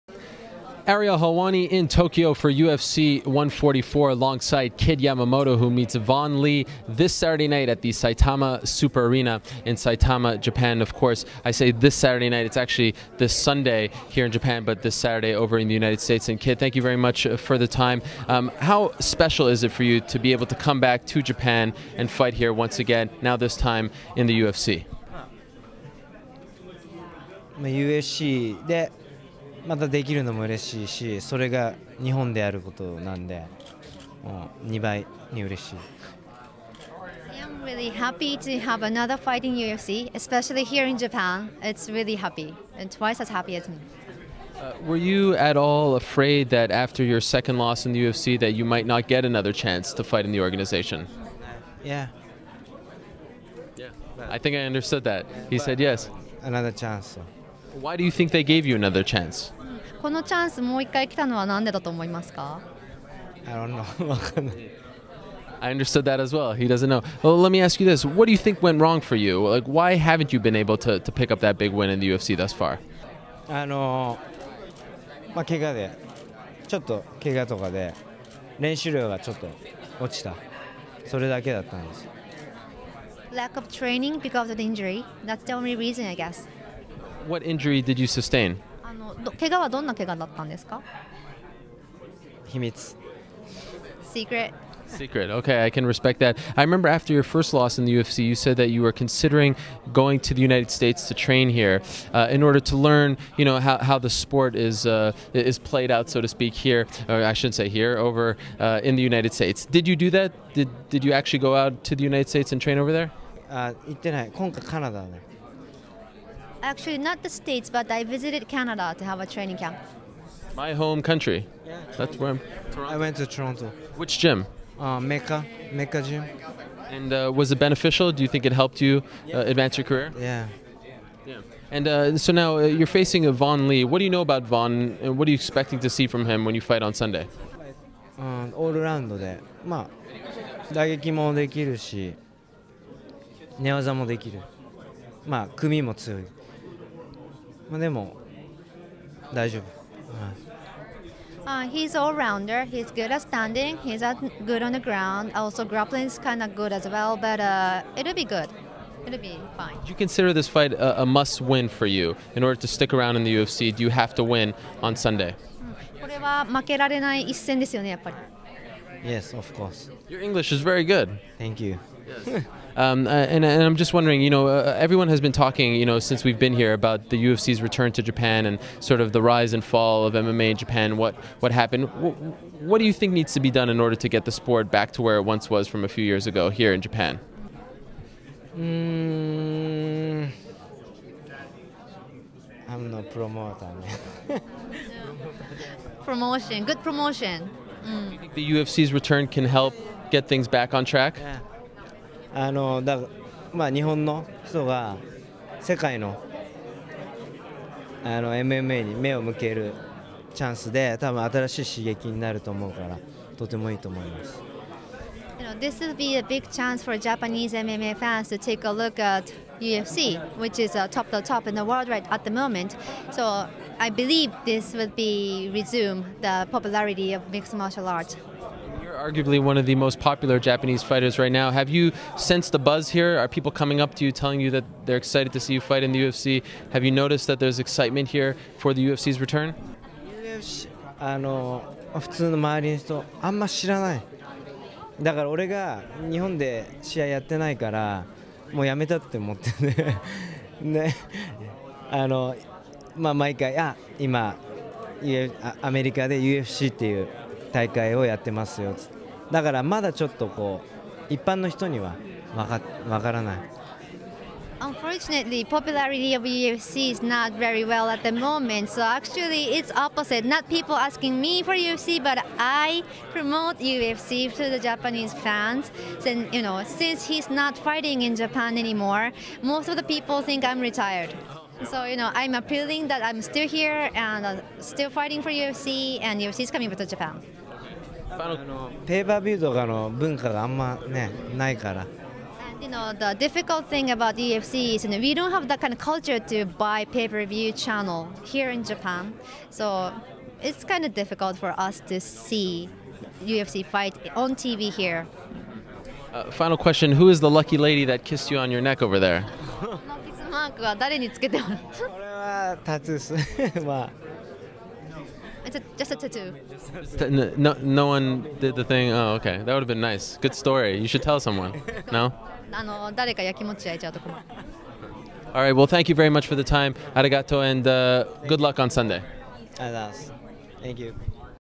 Kid Yamamoto Blames Injury for Struggles in UFC
Kid Yamamoto tells MMA Fighting's Ariel Helwani why he hasn't done well in the UFC and why he hopes to turn it around at UFC 144.